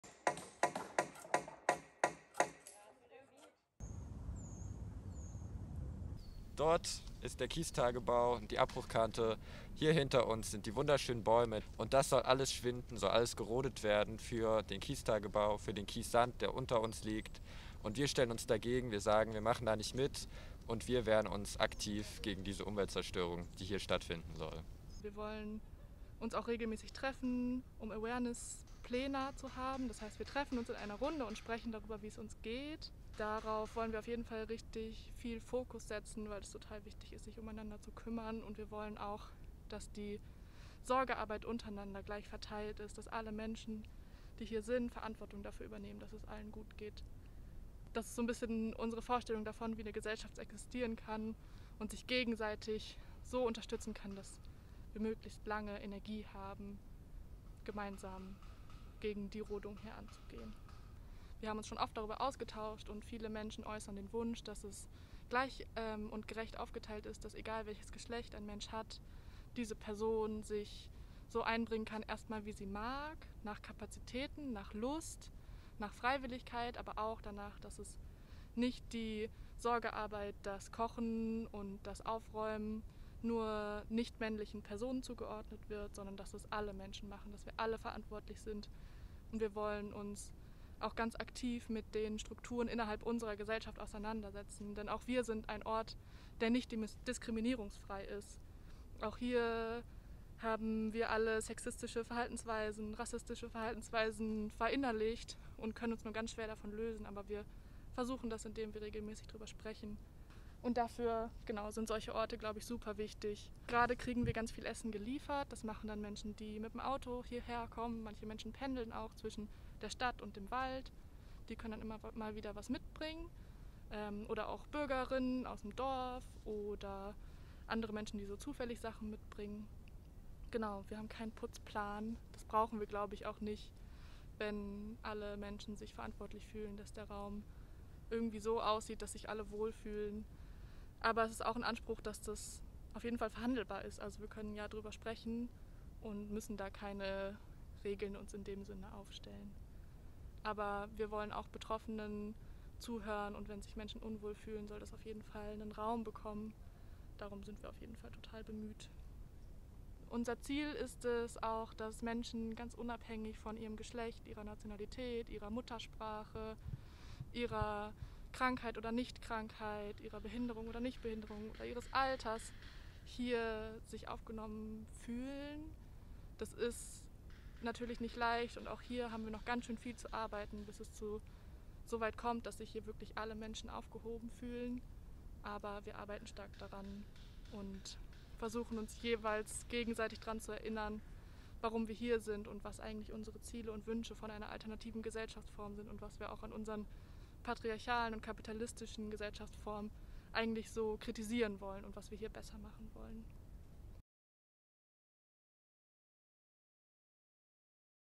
Waldbesetzung bei Dresden: Gefühle, Sorgearbeit, Solidarität, Emanzipation I
Laußnitzer Heide bei Ottendorf-Okrilla, nahe Dresden in Sachsen, 21.8.21
Waldbesetzung HeiboBleibt gegen Waldrodung durch Kieswerk Ottendorf-Okrilla (KBO) für Kiesabbau seit Anfang August 2021, Teil 3